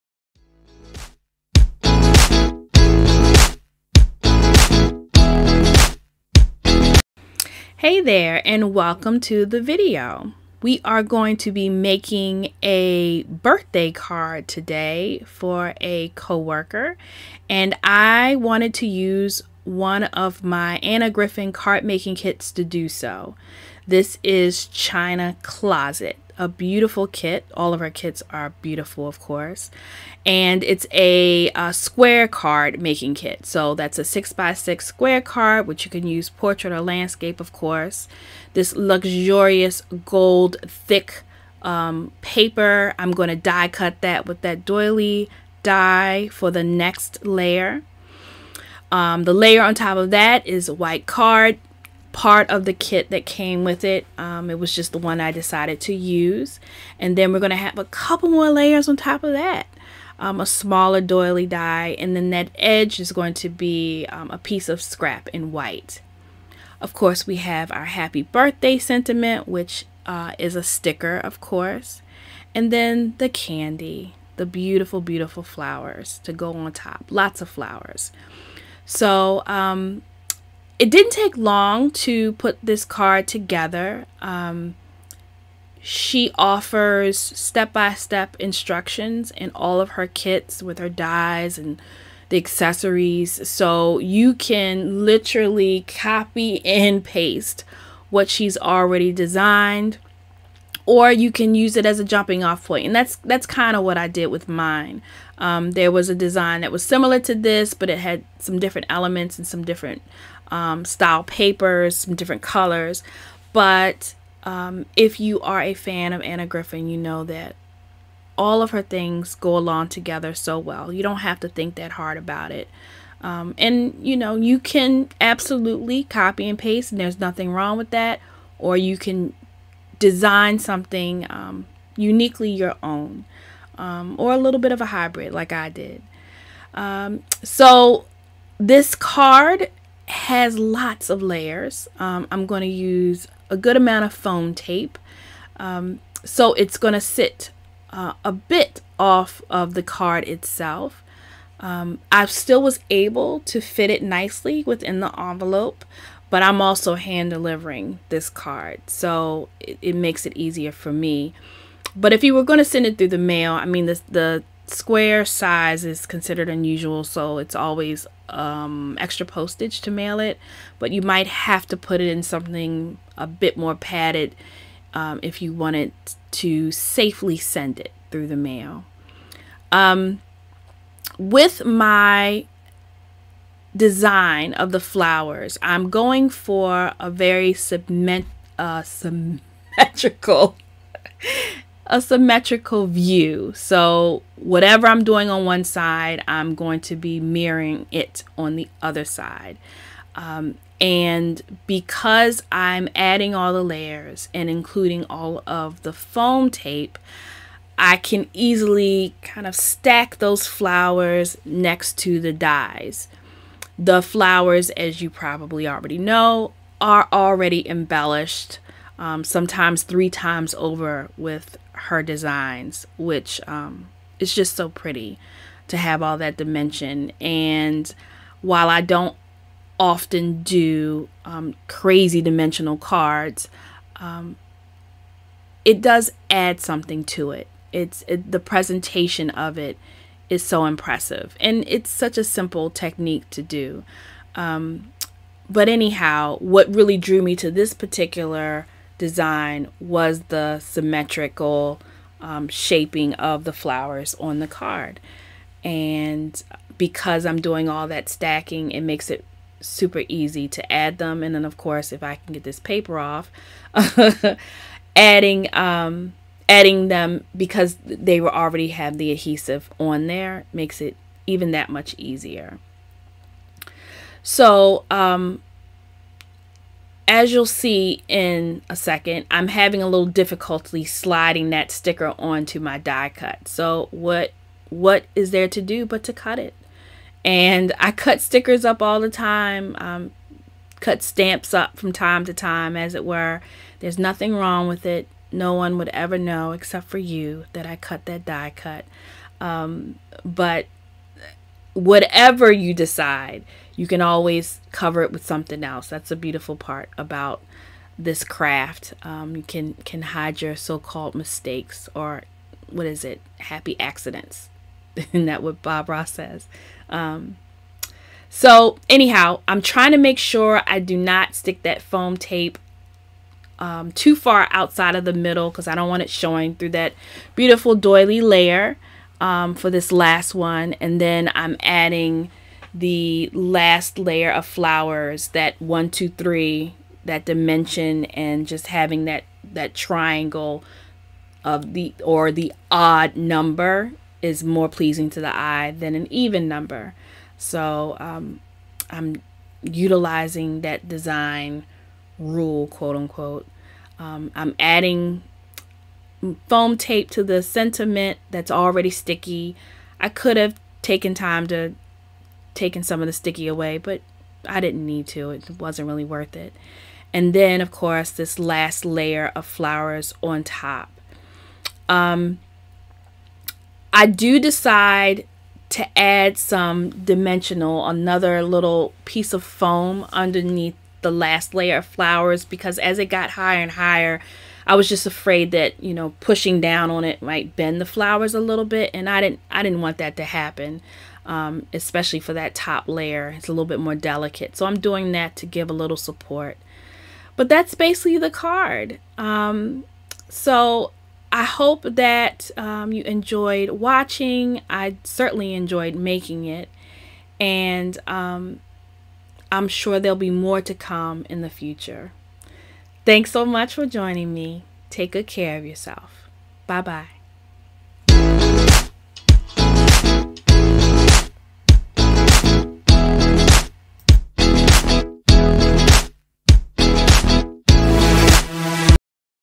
Anna Griffin Card Demo ✂ Lots of Luxurious Layers!
Finally breaking out the Anna Griffin China Closet card making kit. The occasion? A birthday card for a co-worker. Anna's kits are so easy to use when time is short. Everything coordinates perfectly and it's hard to mess up when you follow along with her detailed written tutorials.

Have you ever tried using one of her kits?

We appreciate you stopping by today. Take good care of yourself. ❤️

Shop with Us:

---Etsy Shop